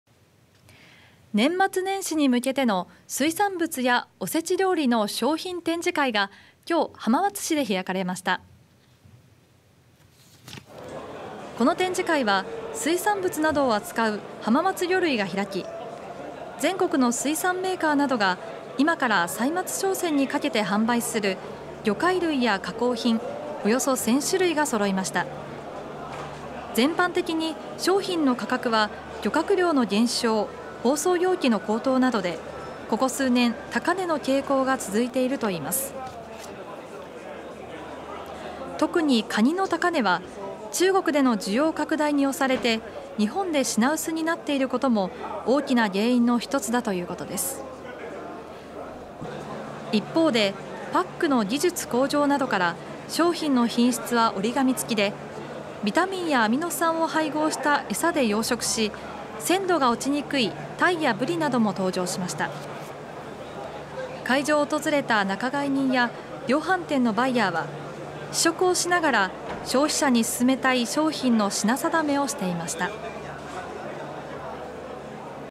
浜松で秋冬・歳末商戦の展示会 年末年始に向け魚介類など１０００種類
年末年始に向けての水産物やおせち料理の商品展示会が１０月８日、浜松市で開かれました。
　この展示会は水産物などを扱う浜松魚類が開き、全国の水産メーカーなどが今から歳末商戦にかけて販売する魚介類や加工品、約１０００種類がそろいました。全般的に商品の価格は漁獲量の減少、包装容器の高騰などで、ここ数年、高値の傾向が続いているといいます。特にカニの高値は、中国での需要拡大に押されて日本で品薄になっていることも大きな原因の一つだということです。一方で、パックの技術向上などから商品の品質は折り紙付きで、ビタミンやアミノ酸を配合したエサで養殖し鮮度が落ちにくいタイやブリなども登場しました。会場を訪れた仲買人や量販店のバイヤーは、試食をしながら消費者に勧めたい商品の品定めをしていました。